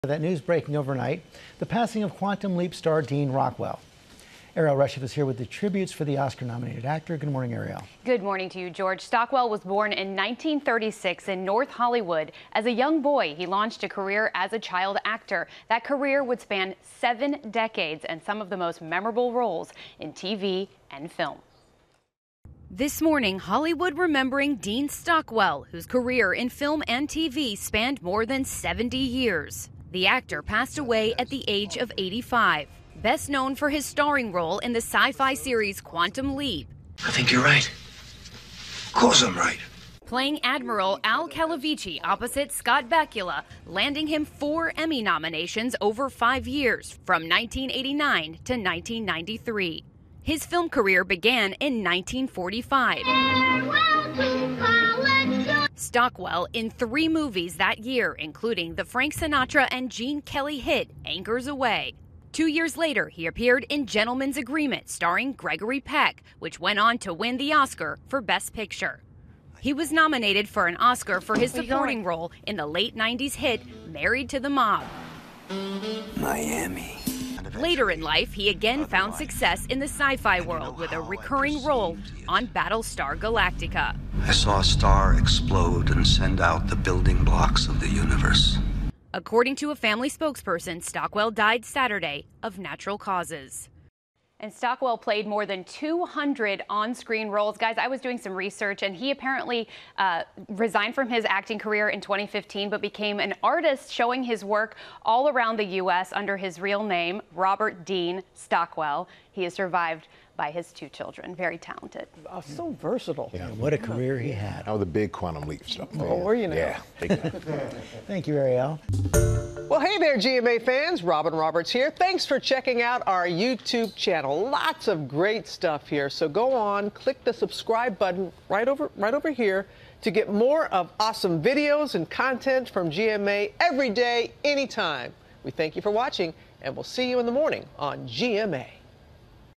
Actor Dean Stockwell dead at 85 l GMA
The actor, whose career in film and TV spanned more than 70 years, was best known for his starring role in the sci-fi series, "Quantum Leap."